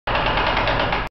trieuse de grains
machine pour trier le grain au Beyrac en Lozère